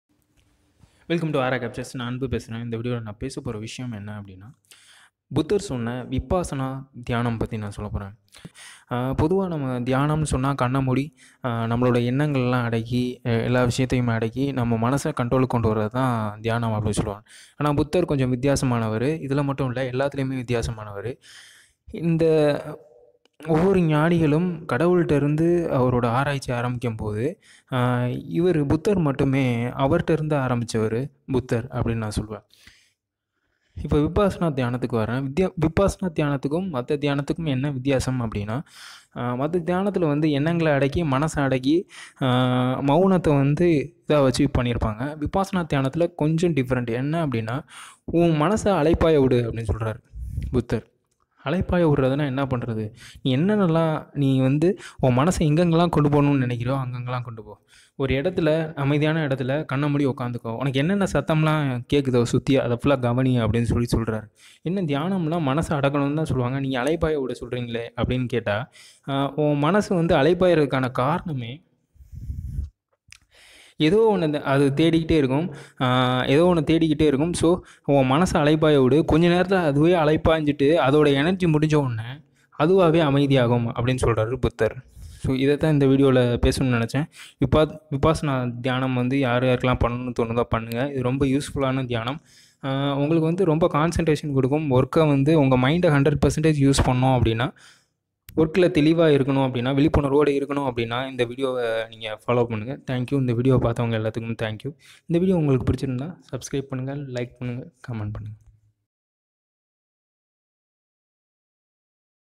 புத்தர் சொன்ன விப்பாசன தியானம்.... | Aaara captures | Anbu talks
இந்த உலகத்தில் தியான முறைகளை வகுத்ததில் புத்தருக்கு மிக முக்கிய பங்கு உண்டு..மற்ற தியான முறைகளை விட புத்த மதத்தில் விபாசனா தியானத்திற்கு மிக முக்கிய பங்கு உண்டு..அந்த விபாசனா தியானத்தை பற்றி ஒரு விளக்கம் இந்த காணொளியில் கொடுக்க பட்டுள்ளது